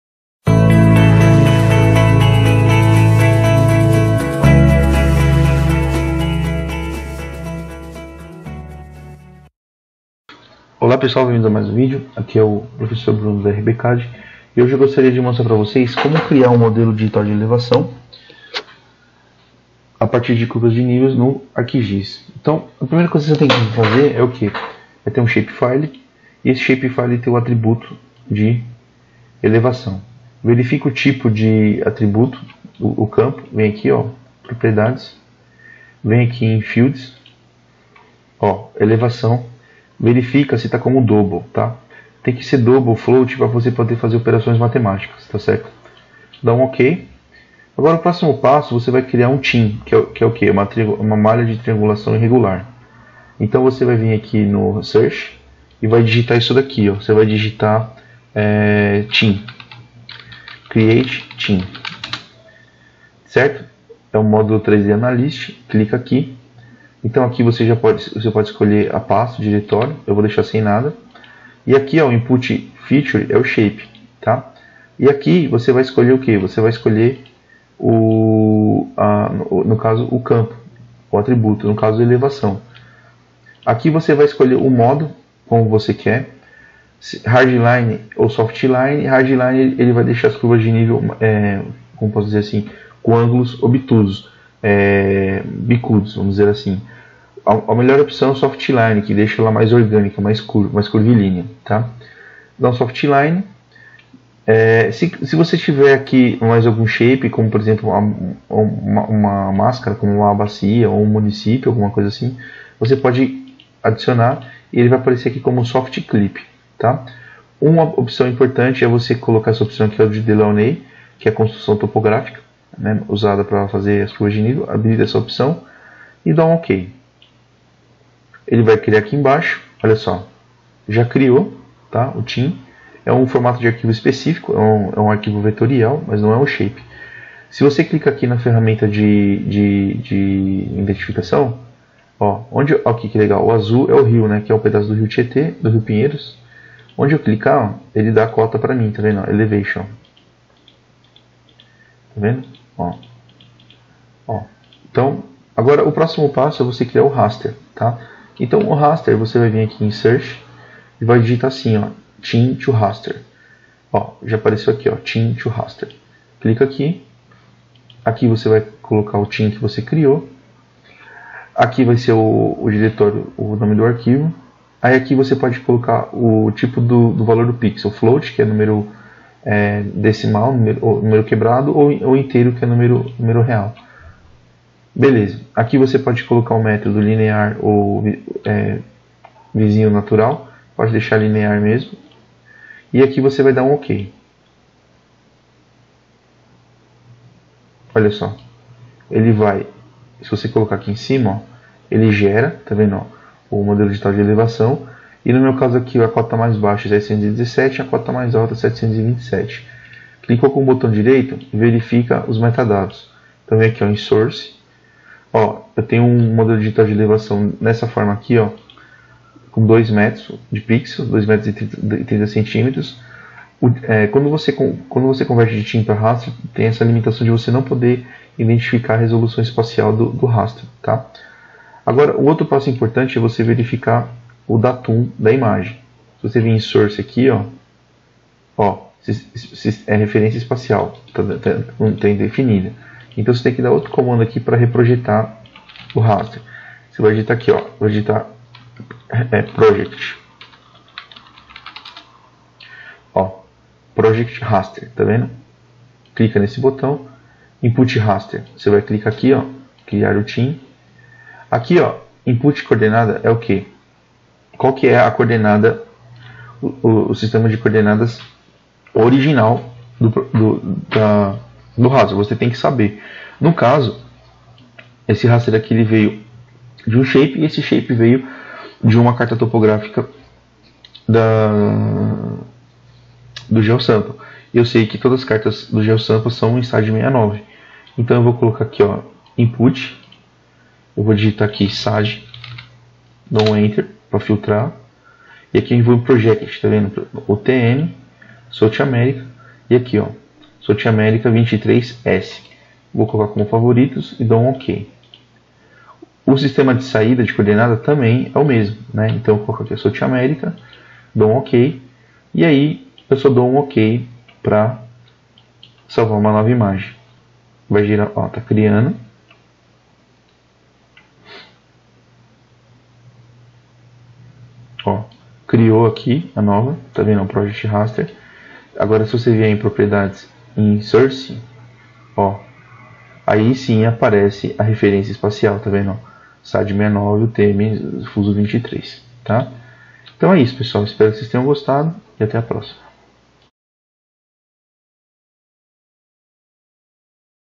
Modelo Digital de Elevação no ArcGIS
Neste vídeo ensino a como criar um modelo digital de elevação no ArcGIS a partir de curvas de nível.